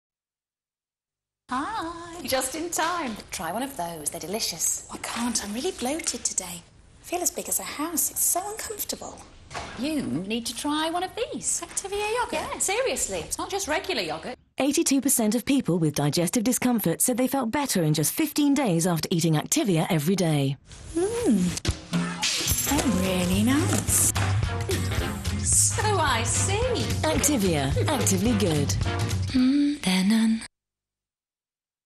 Joanne Heywood in Activia Yoghurt Commercial.MPG
Joanne Heywood in "Hattie & Lou", a TV commercial for Danone's Activia Yoghurt.